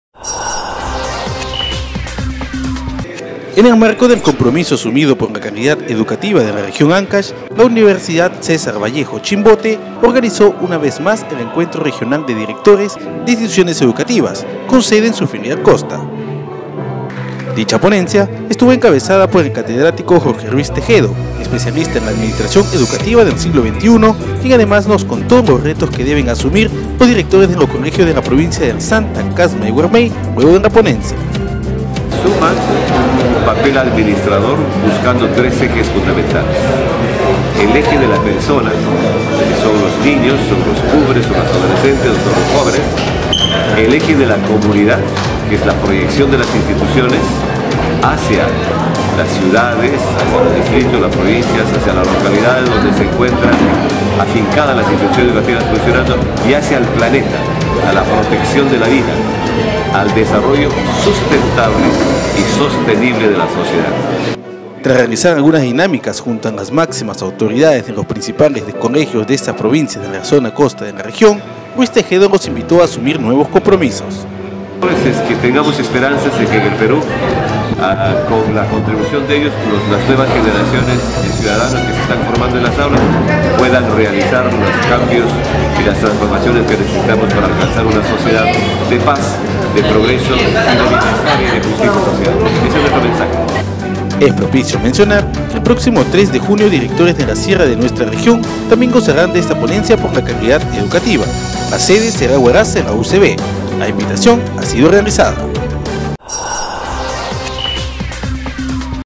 UCV - CHIMBOTE ORGANIZÓ ENCUENTRO REGIONAL DE DIRECTORES DE COLEGIOS
Como parte de su compromiso con la calidad educativa del país, la UCV – Chimbote organizó el Encuentro Regional de Directores 2016, en su campus universitario.